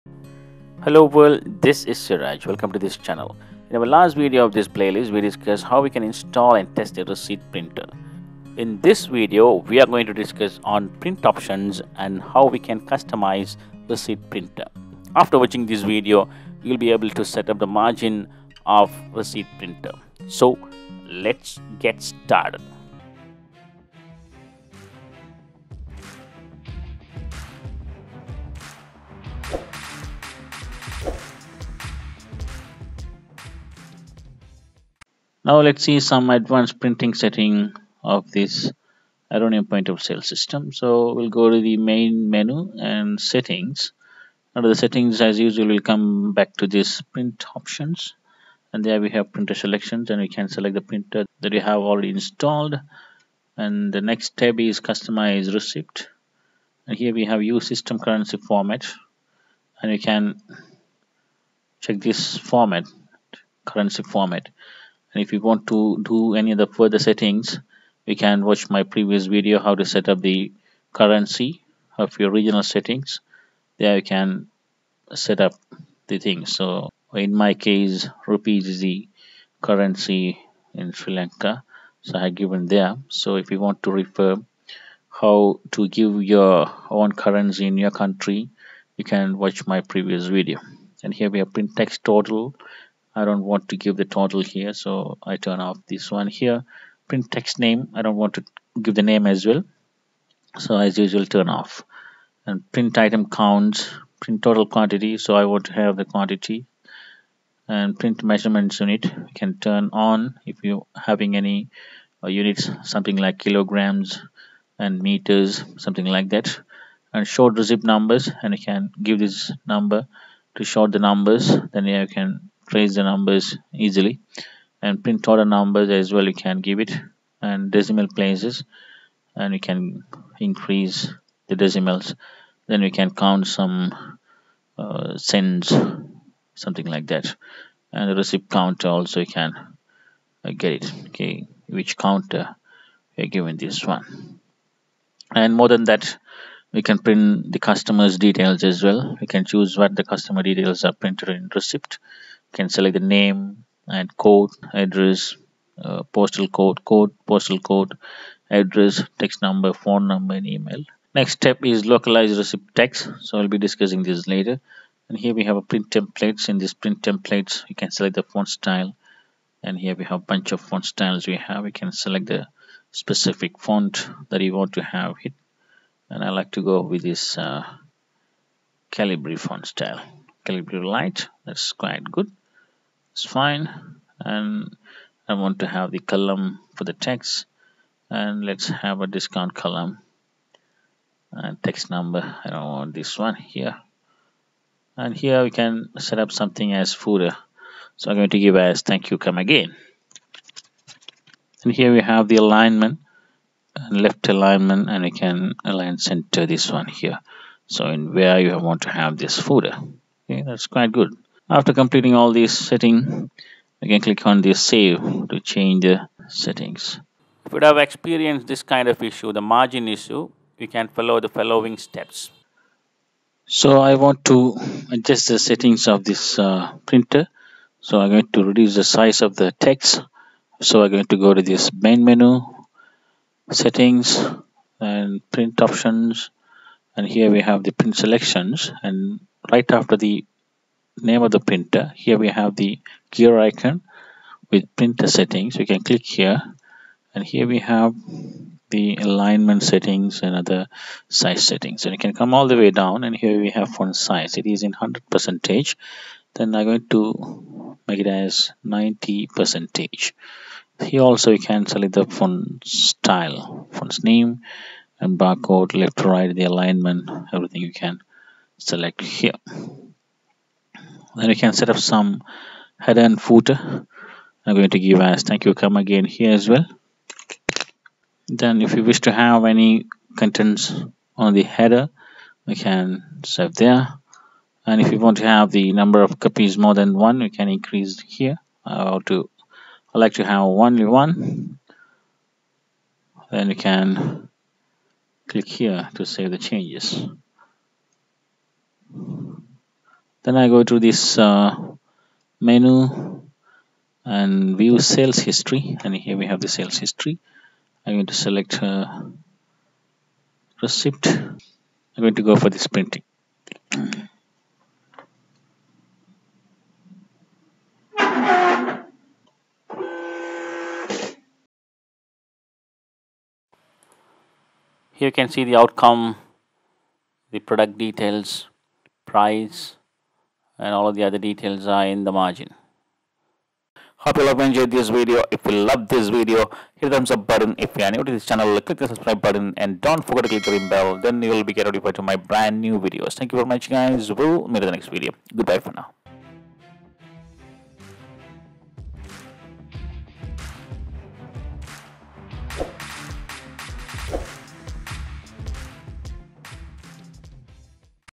Chapter 18 POS CUSTOMIZE RECEIPT PRINTER EPSON Aronium Best and Free POS
Here you have the video on setup POS receipt printer in general and Aronium POS. This is just setup video and I will be publishing the full settings including alignment video in the next video of this playlist.